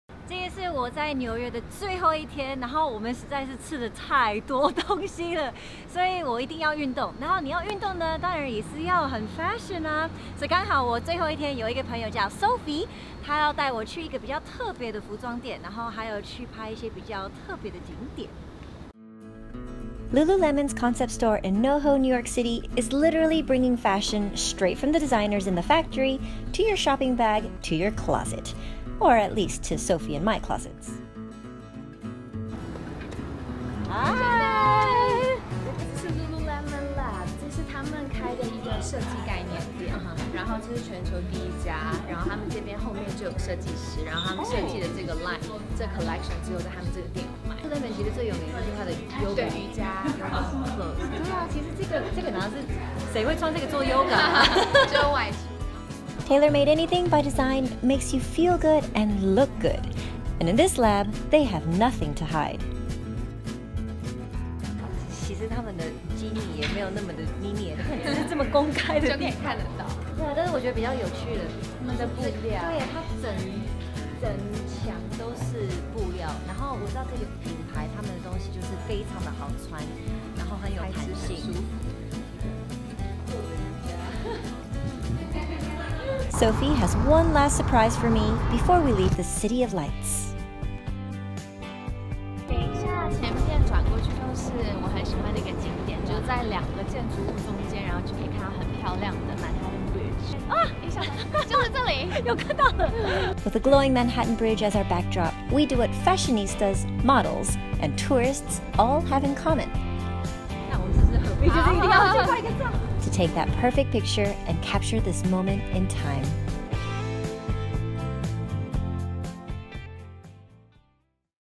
謝怡芬Janet 的紐約城市好朋友Sophie | 人物特寫 | Vogue Taiwan
謝怡芬Janet 及她的城市好朋友Sophie一同在紐約幫大家介紹一間很有名的lululemon lab，裡頭結合運動及時尚的服飾都很特別～最後還有一個Sophie的紐約私房景點. 快跟著謝怡芬Janet 和Sophie的腳步一起走吧！

√ 素人改造3步驟！化身超正女孩非難事
√ 瘦腿、塑腰、練翹臀！最多減肥資訊看這裡